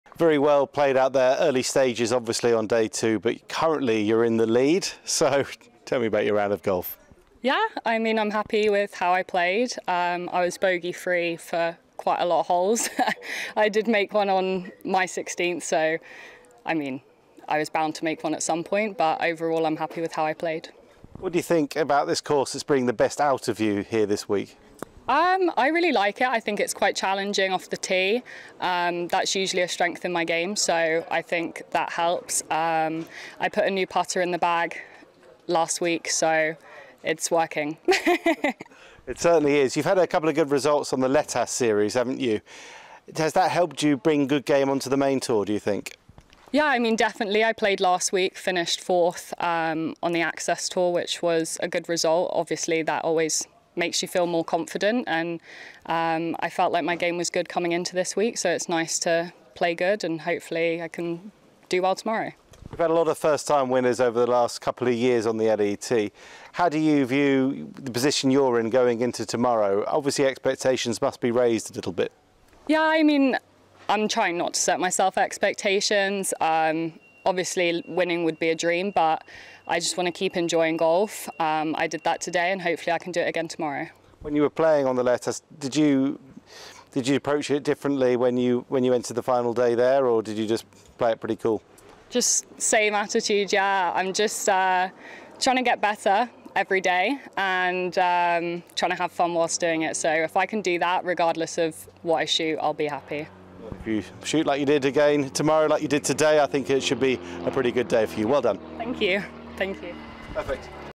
Amy Taylor fires a stunning 67 (-5) at Golf Nazionale | Ladies Italian Open
England's Amy Taylor posted a superb second round 67 (-5) to make a big move in Italy and setting up the chance to win her maiden LET title